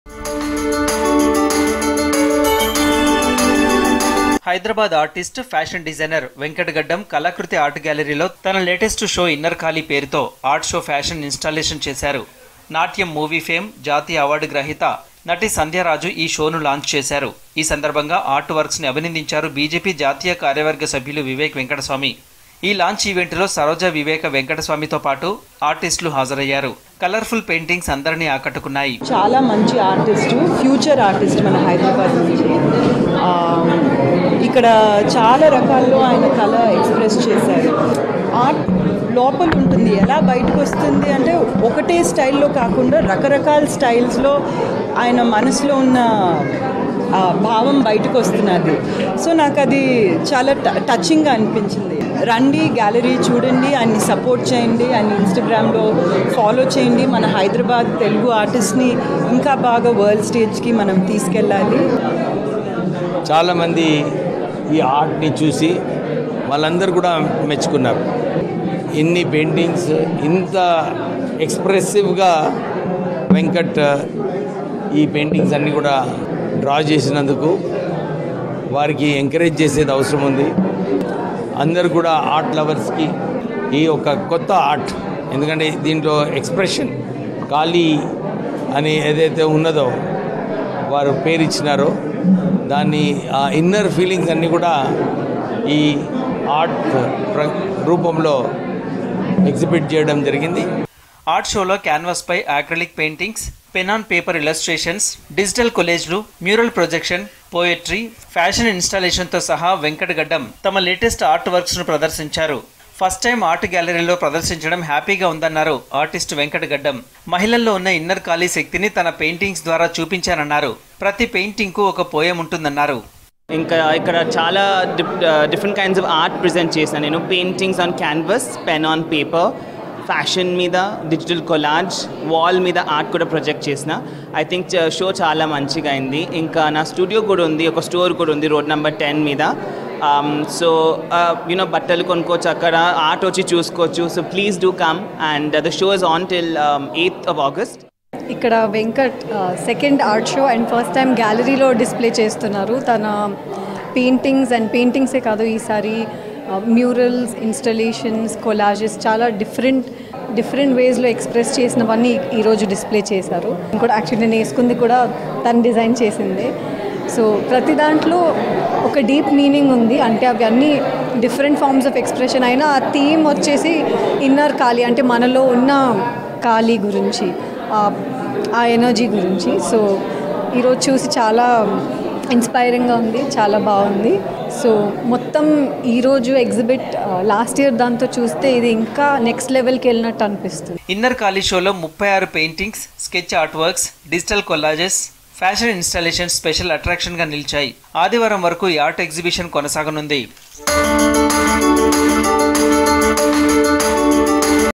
Actress Sandhya Raju Launches Kalakriti Art Gallery Show | Venkat Gaddam | Vivek Venkataswamy | V6
Actress Sandhya Raju Launches Kalakriti Art Gallery Show | Venkat Gaddam | Vivek Venkataswamy | V6 News